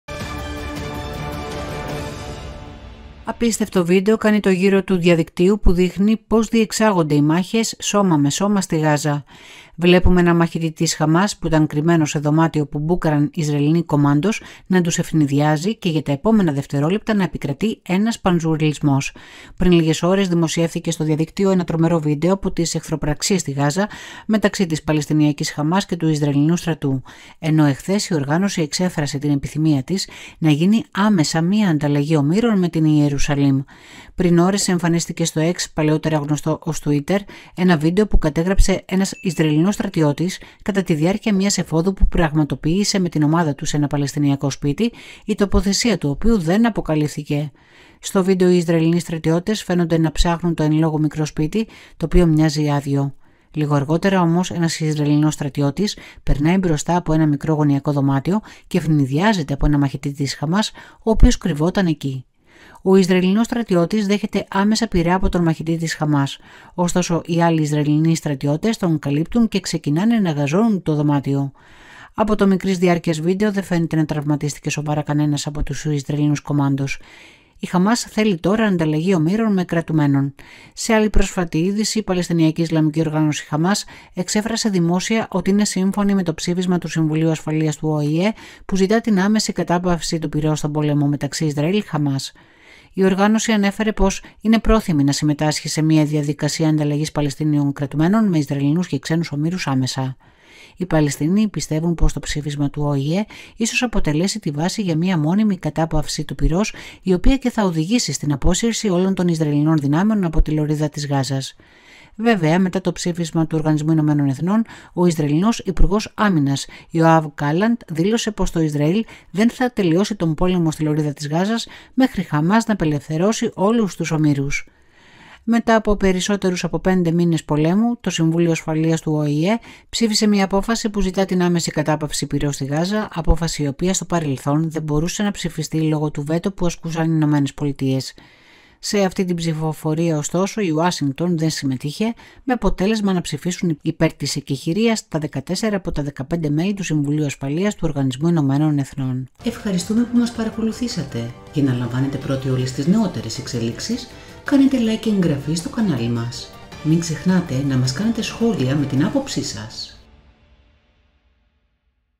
Συγκλονιστικό βίντεο: 9 δευτερόλεπτα πανζουρλισμού με μαχητή της Χαμάς να αιφνιδιάζει Ισραηλινούς
Απίστευτο βίντεο κάνει το γύρο του διαδικτύου που δείχνει πως διεξάγονται οι μάχες σώμα με σώμα στη Γάζα. Βλέπουμε έναν μαχητή της Χαμάς που ήταν κρυμμένος σε δωμάτιο που μπούκαραν Ισραηλινοί κομάντος να τους αιφνιδιάζει και για τα επόμενα δευτερόλεπτα να επικρατεί ένας πανζουρλισμός.

Στο βίντεο οι Ισραηλινοί στρατιώτες φαίνονται να ψάχνουν το εν λόγω μικρό σπίτι, το οποίο μοιάζει άδειο. Λίγο αργότερα όμως ένας Ισραηλινός στρατιώτης περνάει μπροστά από ένα μικρό γωνιακό δωμάτιο και αιφνιδιάζεται από έναν μαχητή της Χαμάς ο οποίος κρύβονταν εκεί.

Ο Ισραηλινός στρατιώτης δέχεται άμεσα πυρά από τον μαχητή της Χαμάς, ωστόσο οι άλλοι Ισραηλινοί στρατιώτες τον καλύπτουν και ξεκινάνε να γαζώνουν το δωμάτιο.

Κεφάλαια:
0:00 - Εισαγωγή
0:03 - Μαχητής της Χαμάς εφνιδιαζει Ισραηλινούς
1:16 - Η Χαμάς Θέλει Τώρα Ανταλλαγή Ομήρων με Κρατουμένων

Παράλληλα, η Χαμάς ζήτησε άμεση εκεχειρία και ανταλλαγή Ισραηλινών ομήρων με Παλαιστίνιους κρατούμενους.

Πριν λίγες ώρες δημοσιεύθηκε στο διαδίκτυο ένα τρομερό βίντεο από τις εχθροπραξίες στην Γάζα μεταξύ της παλαιστινιακής Χαμάς και του ισραηλινού στρατού, ενώ εχθές η οργάνωση εξέφρασε την επιθυμία της να γίνει άμεσα μία ανταλλαγή ομήρων με την Ιερουσαλήμ.

Το Απίστευτο Βίντεο

Πριν ώρες εμφανίστηκε στο X, παλαιότερα γνωστό ως Twitter, ένα βίντεο που κατέγραψε ένας Ισραηλινός στρατιώτης κατά την διάρκεια μίας εφόδου που πραγματοποίησε με την ομάδα του σε ένα παλαιστινιακό σπίτι, η τοποθεσία του οποίου δεν αποκαλύφθηκε.

Στο βίντεο οι Ισραηλινοί στρατιώτες φαίνονται να ψάχνουν το εν λόγω μικρό σπίτι, το οποίο μοιάζει άδειο. Λίγο αργότερα όμως ένας Ισραηλινός στρατιώτης περνάει μπροστά από ένα μικρό γωνιακό δωμάτιο και αιφνιδιάζεται από έναν μαχητή της Χαμάς ο οποίος κρύβονταν εκεί. Ο Ισραηλινός στρατιώτης δέχεται άμεσα πυρά από τον μαχητή της Χαμάς, ωστόσο οι άλλοι Ισραηλινοί στρατιώτες τον καλύπτουν και ξεκινάνε να γαζώνουν το δωμάτιο.

Από το μικρής διάρκειας βίντεο δεν φαίνεται να τραυματίστηκε σοβαρά κανένας από τους Ισραηλινούς κομάντος.

Η Χαμάς Θέλει Τώρα Ανταλλαγή Ομήρων με Κρατουμένων

Σε άλλη πρόσφατη είδηση, η παλαιστινιακή ισλαμική οργάνωση Χαμάς εξέφρασε δημόσια ότι είναι σύμφωνη με το ψήφισμα του Συμβουλίου Ασφαλείας του ΟΗΕ που ζητά την «άμεση κατάπαυση του πυρός» στον πόλεμο μεταξύ Ισραήλ-Χαμάς. Η οργάνωση ανέφερε πως είναι πρόθυμη να συμμετάσχει σε μία διαδικασία ανταλλαγής Παλαιστίνιων κρατουμένων με Ισραηλινούς και ξένους ομήρους άμεσα.

Οι Παλαιστίνιοι πιστεύουν πως το ψήφισμα του ΟΗΕ ίσως αποτελέσει την βάση για μία μόνιμη κατάπαυση του πυρός, η οποία και θα οδηγήσει στην απόσυρση όλων των ισραηλινών δυνάμεων από την Λωρίδα της Γάζας.

Βέβαια μετά το ψήφισμα του ΟΗΕ, ο Ισραηλινός υπουργός άμυνας Ιωάβ Γκάλαντ δήλωσε πως το Ισραήλ δεν θα τελειώσει τον πόλεμο στην Λωρίδα της Γάζας μέχρι η Χαμάς να απελευθερώσει όλους τους ομήρους.

Μετά από περισσότερους από πέντε μήνες πολέμου, το Συμβούλιο Ασφαλείας του ΟΗΕ ψήφισε μία απόφαση που ζητά την «άμεση κατάπαυση πυρός» στην Γάζα, απόφαση η οποία στο παρελθόν δεν μπορούσε να ψηφιστεί λόγω του βέτο που ασκούσαν οι Ηνωμένες Πολιτείες. Σε αυτήν την ψηφοφορία ωστόσο η Ουάσιγκτον δεν συμμετείχε με αποτέλεσμα να ψηφίσουν υπέρ της εκεχειρίας τα 14 από τα 15 μέλη του Συμβουλίου Ασφαλείας του ΟΗΕ.